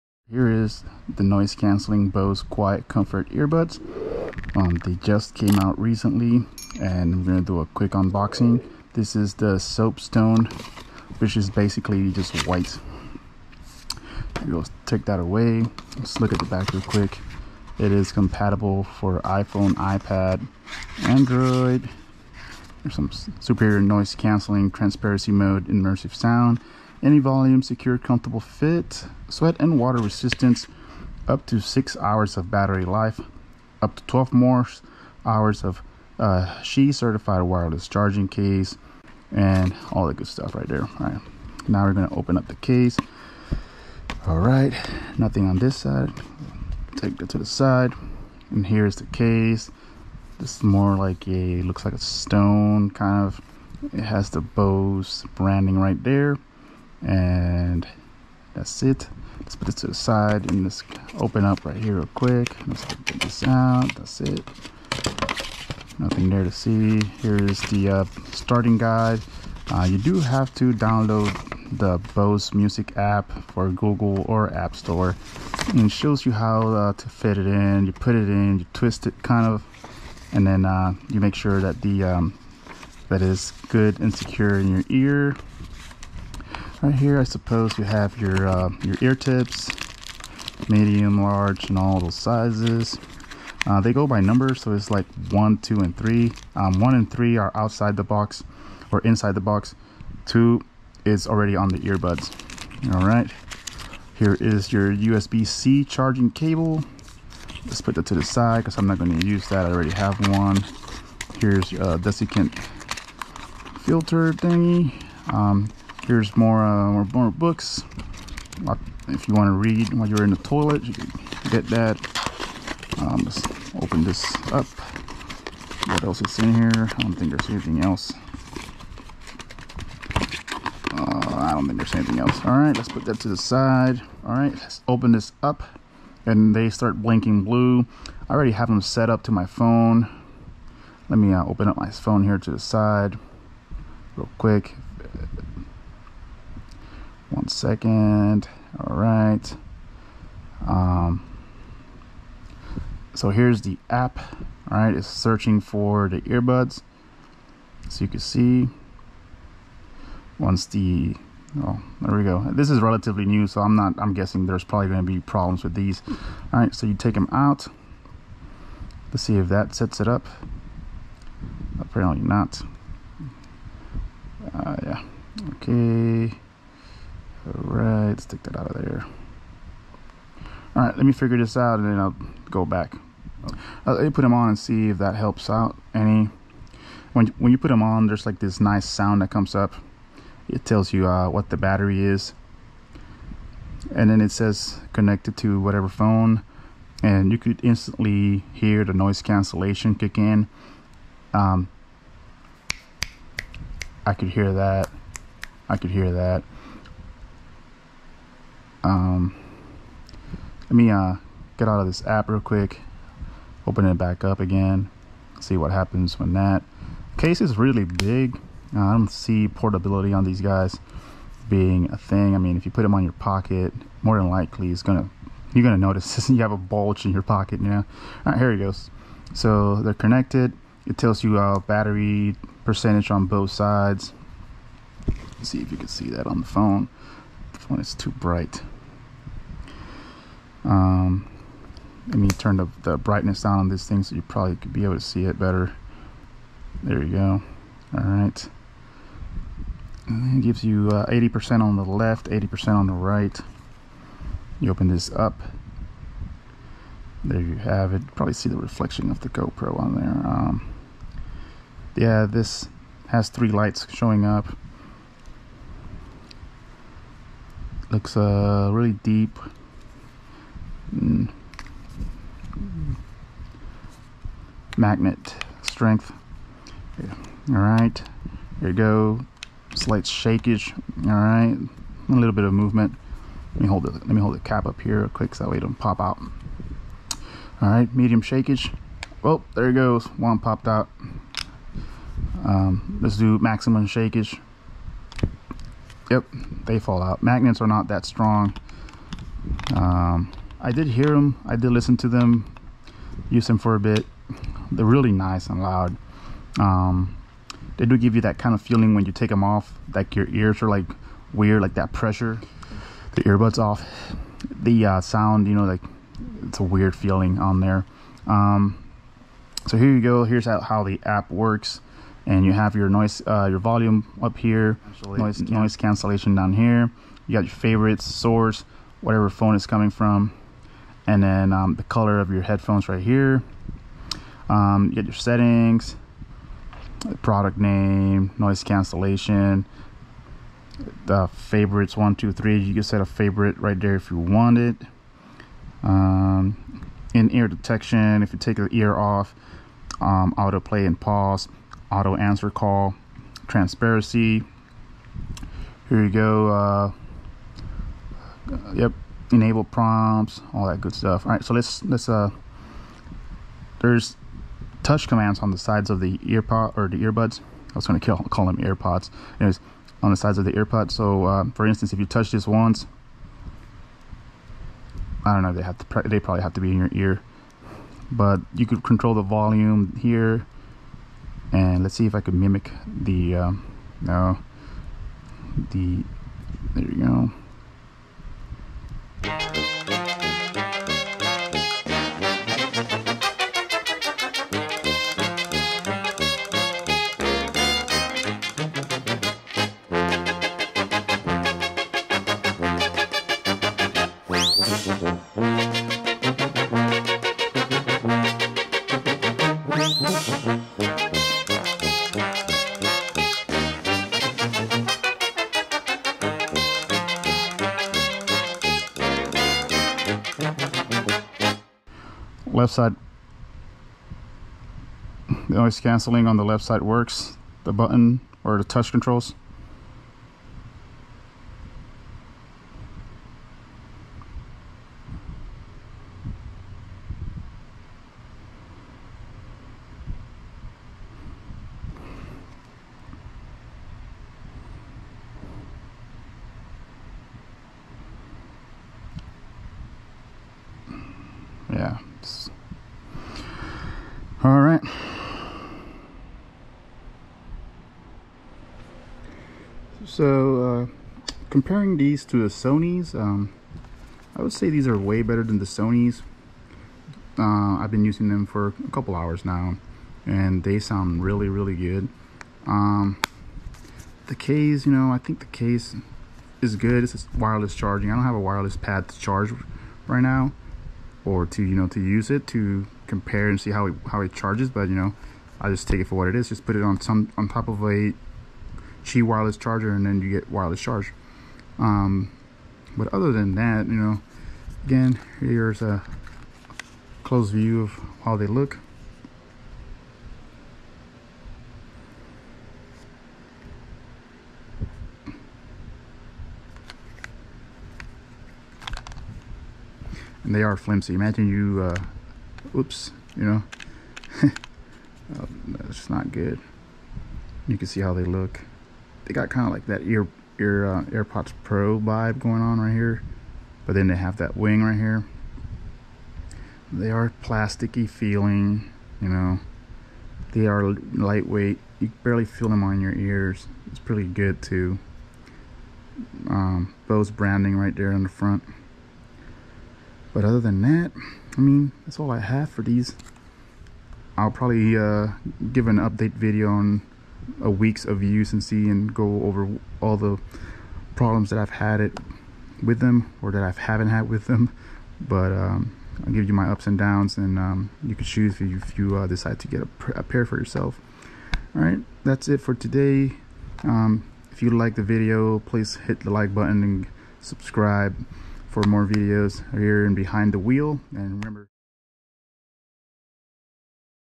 Bose QC earbuds AirPod Pro Killers? Unboxing and Review. Read description eartips may hurt!
This is a real review. I purchase these . **Update 3** AirPods are better at comfort but Bose QC earbuds are better at sound! **Update 2** Started the return they are hurt my ears. Your experience might be different than mine as everyone has a different ear type. The sound is good but again the pain is not. Kept taking them off and on to sooth the pain. Buyers beware **Update 1** I’m highly considering returning these as they hurt after a good use. Changed eartips and all with no luck. They at first feel nice but after a while they start to hurt. Unboxing and initial impression review on Bose new QC Noise Cancellation earbuds.